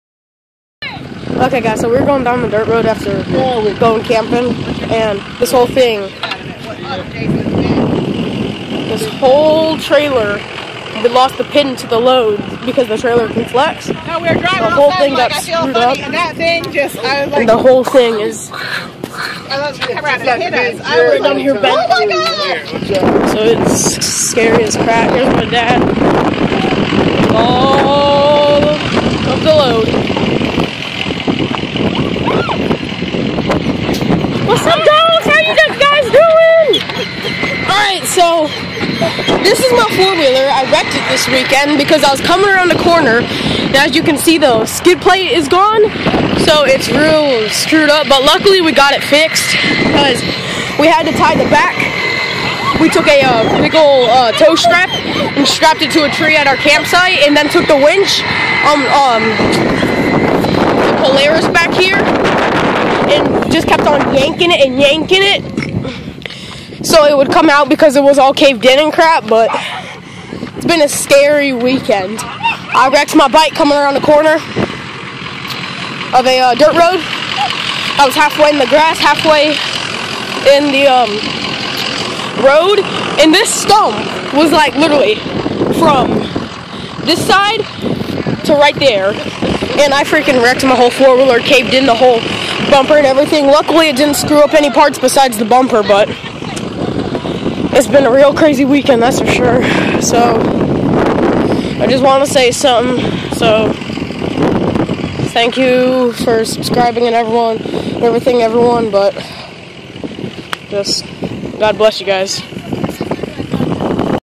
DANGEROUS Weekend 3/24-3/26/17
From wrecking my four wheeler to having a fish tailing trailer.